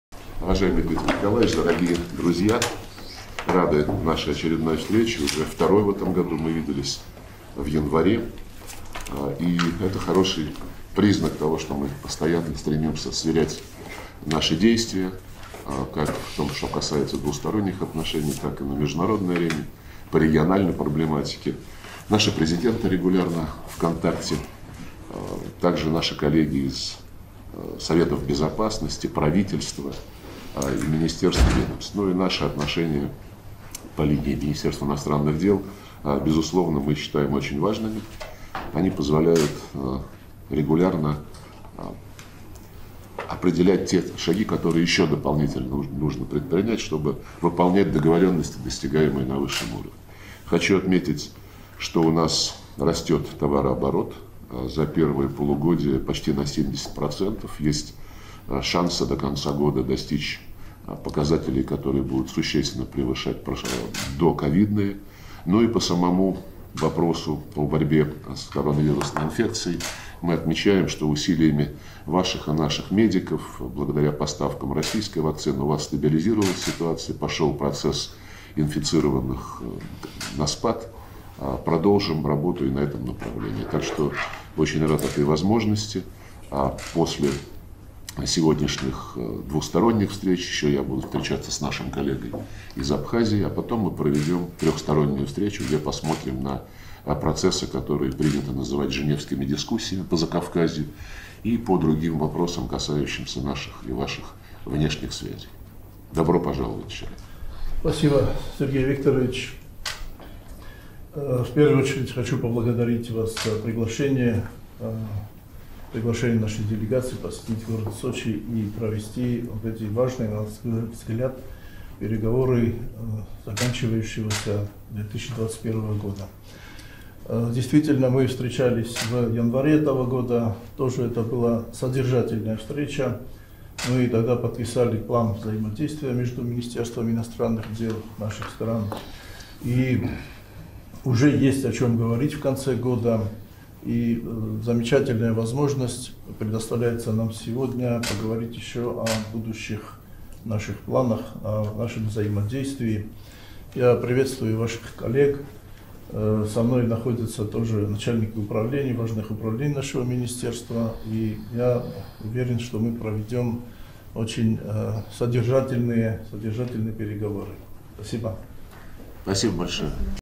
С.Лавров и Д.Медоев, Сочи, 20 декабря 2021 года
Вступительное слово Министра иностранных дел Российской Федерации С.В.Лаврова в ходе встречи с Министром иностранных дел Южной Осетии Д.Н.Медоевым, Сочи, 20 декабря 2021 года